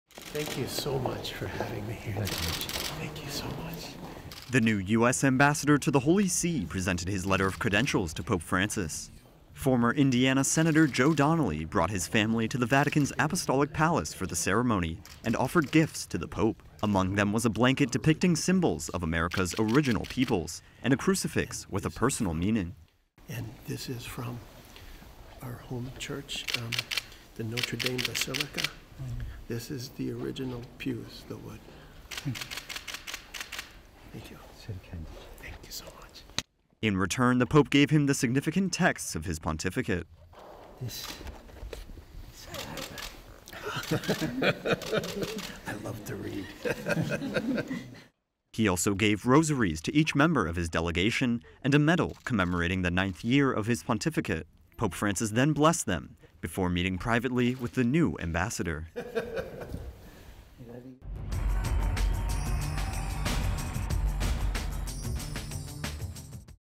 New U S ambassador meets PopeFrancis at the Vatican
JoeDonnelly, the new U.S. ambassador to the HolySee, presented his letter of credentials to PopeFrancis at the Vatican's Apostolic Palace.

He was accompanied by his family and offered gifts to the Pope, including a blanket depicting symbols of America's original peoples and a crucifix with a personal meaning. In return, Pope Francis gave Donnelly significant texts of his pontificate, rosaries for each member of his delegation, and a medal commemorating the ninth year of his pontificate.

The Pope then blessed them and had a private meeting with the new ambassador.